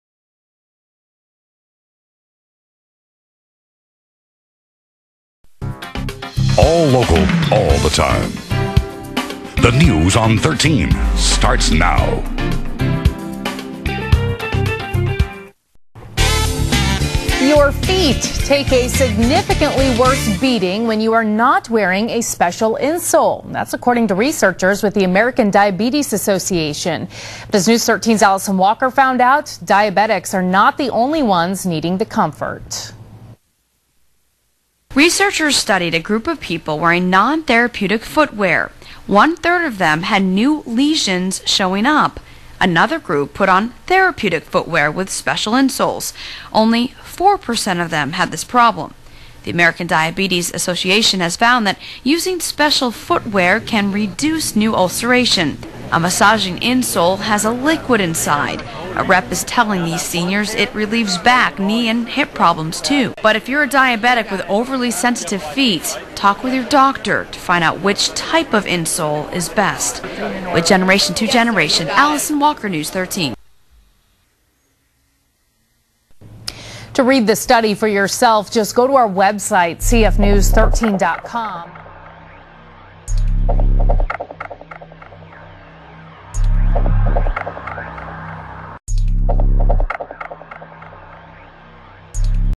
VTS_01_1.VOB
Channel 13 describes how Therapeutic Insoles (Massaging Insoles) relieve foot, leg and back pain and benefit Diabetes sufferers by increasing circulation.